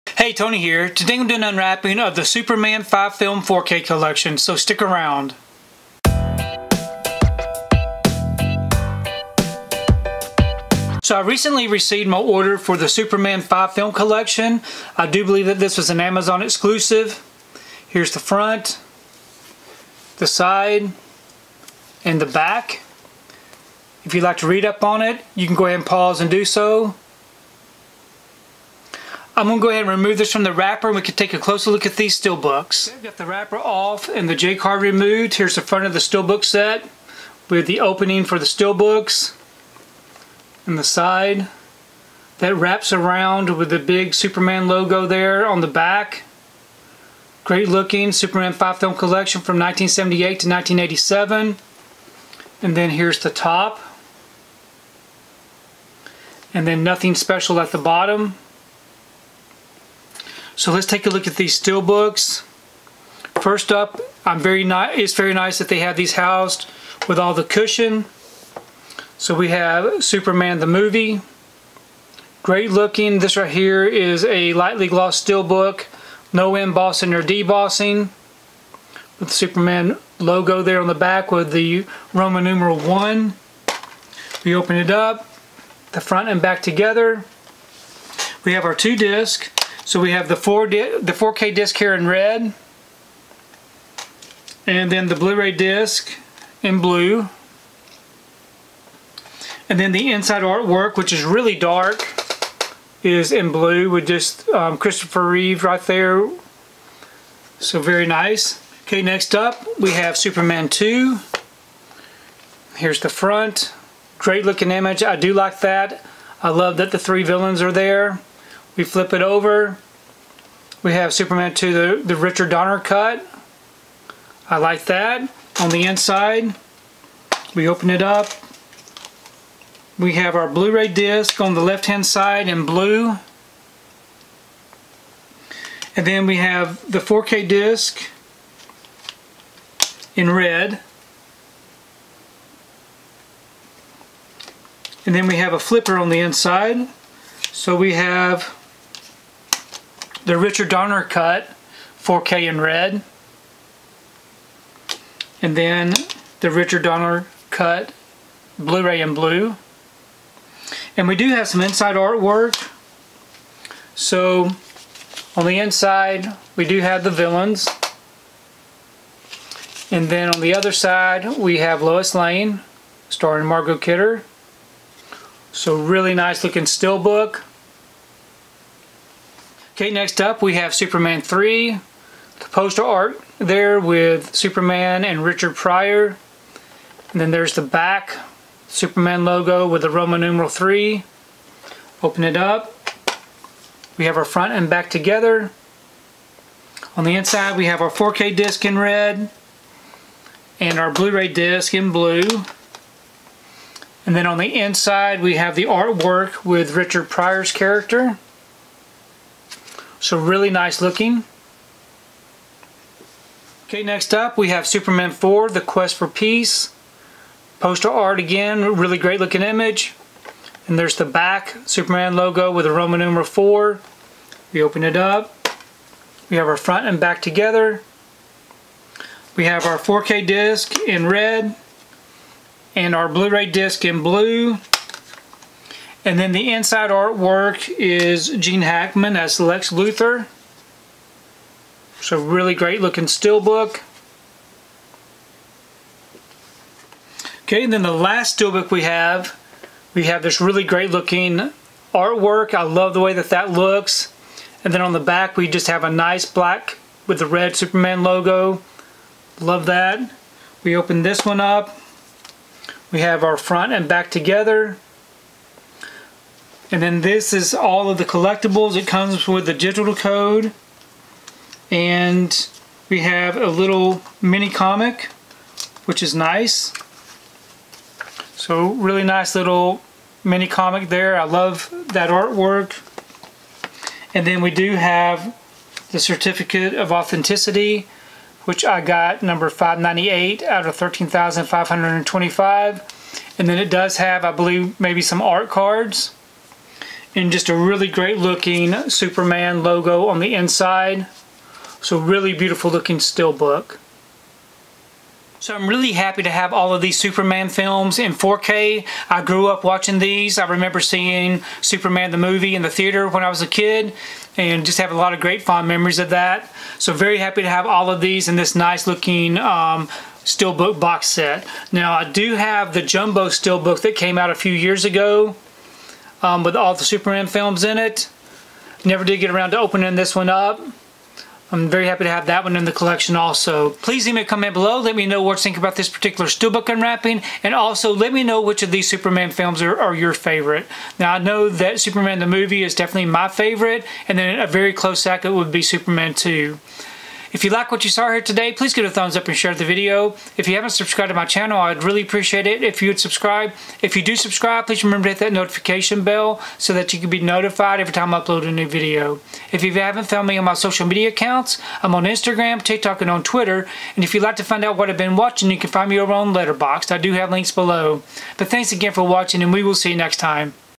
Superman 5-Film Amazon Exclusive / 4K Ultra HD + Blu-ray + Digital / Steelbook Case Collection
Check out my unwrapping of the Superman 5-Film Amazon Exclusive / 4K Ultra HD + Blu-ray + Digital / Steelbook Case Collection!

► Purchase the Superman Steelbook 4K Collection
► Purchase the Superman I-IV 5-Film 4K Collection